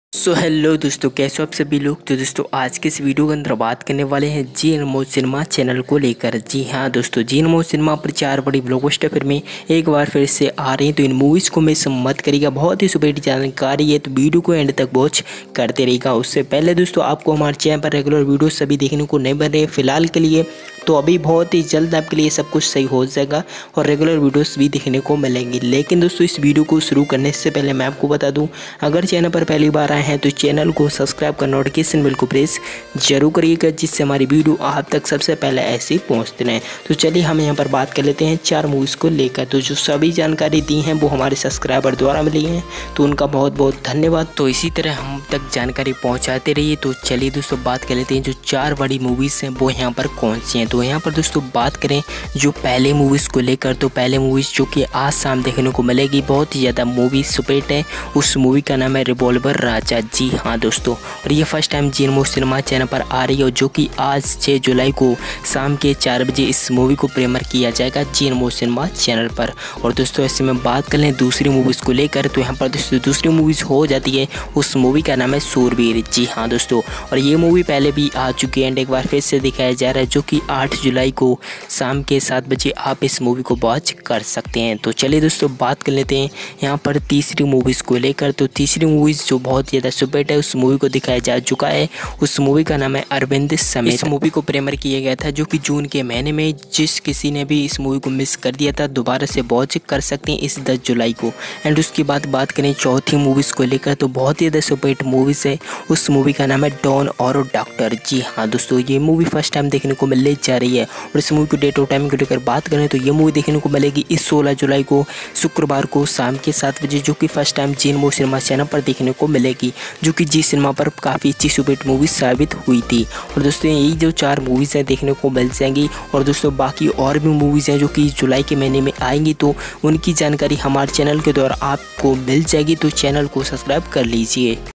Zee Anmol Cinema Par Paheli Baar 4 New Blockbuster Movies This July | DD Free Dish
1 LIKE = MORE MOTIVATION 👍

Fallow On Me 🔥